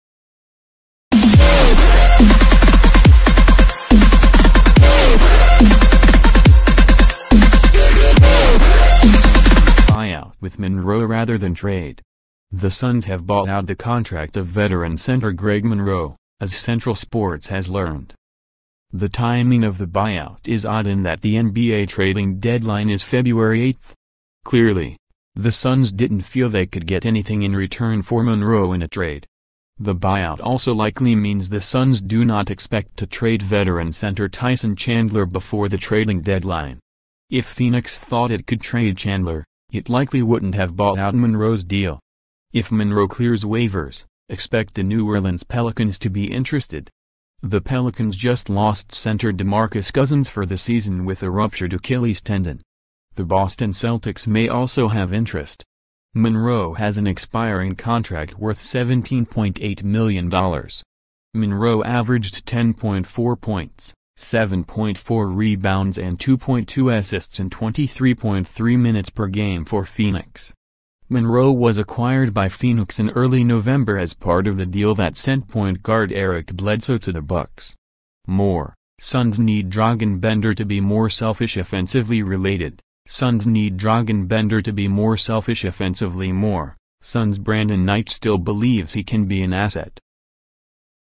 Suns reach buyout with Monroe rather than trade
The Suns have bought out the contract of veteran center Greg Monroe, azcentral sports has learned.

The timing of the buyout is odd in that the NBA trading deadline is Feb. 8. Clearly, the Suns didn't feel they could get anything in return for Monroe in a trade.

The buyout also likely means the Suns do not expect to trade veteran center Tyson Chandler before the trading deadline. If Phoenix thought it could trade Chandler, it likely wouldn't have bought out Monroe's deal.

If Monroe clears waivers, expect the New Orleans Pelicans to be interested. The Pelicans just lost center DeMarcus Cousins for the season with a ruptured Achilles' tendon. The Boston Celtics may also have interest.